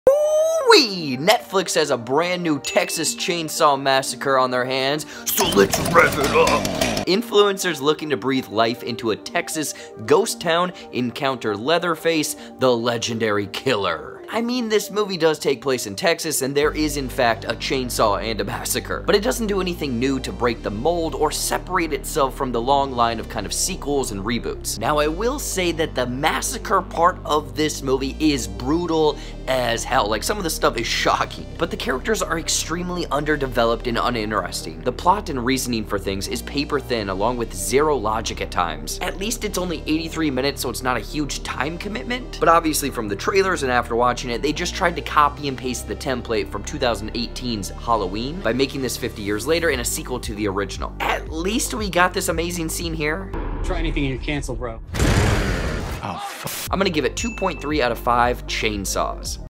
TEXAS CHAINSAW MASSACRE 2022 MOVIE REVIEW
Here are my thoughts on the latest horror slasher movie from Netflix, TEXAS CHAINSAW MASSACRE.

Synopsis: Influencers looking to breathe new life into a Texas ghost town encounter Leatherface, the legendary killer who wears a mask of human skin.